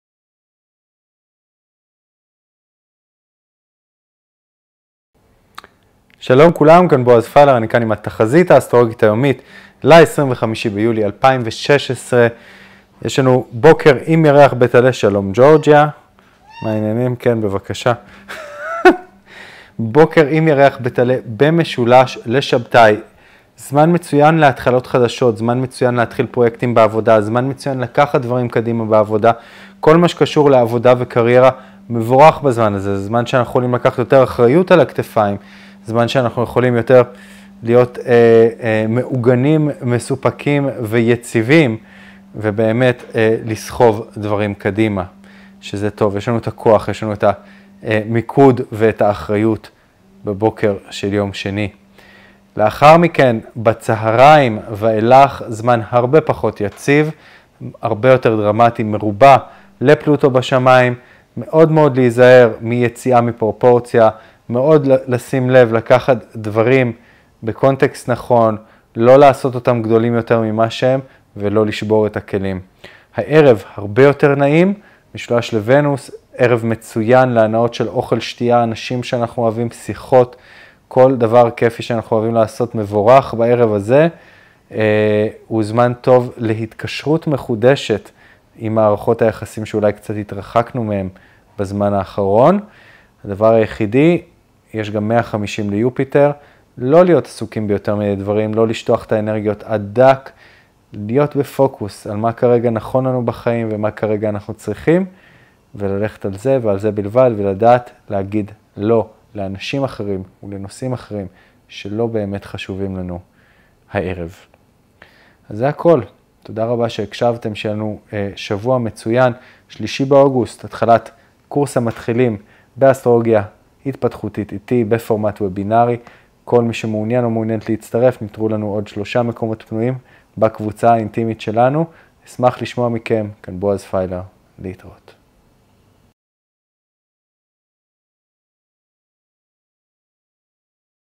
בוקר מעולה לעבודה, צהריים סוער, ערב נעים: התחזית האסטרולוגית היומית עם בועז פיילר 25 יולי 2016
את המשולש לשבתאי בבוקר אנו יכולים להתקדמות בעבודה ופיתוח הקריירה. הצהריים סוערים ויש לשים לב לא לצאת מפרופורציות. הערב יכול להיות מאוד נעים לפעילות משותפת ולכל מה שקשור בהנאות חושיות.